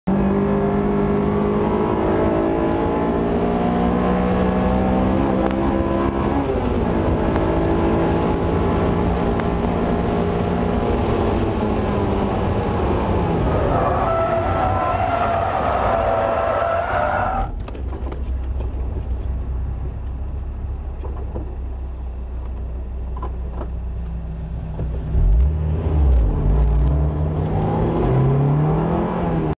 BRAKE! Then Turn...
Harris Hill Raceway 6/15/16 - SCCA Track Day in America
This spin allowed me to meet with the legendary Andy Hollis for a short driver coaching session on pit lane.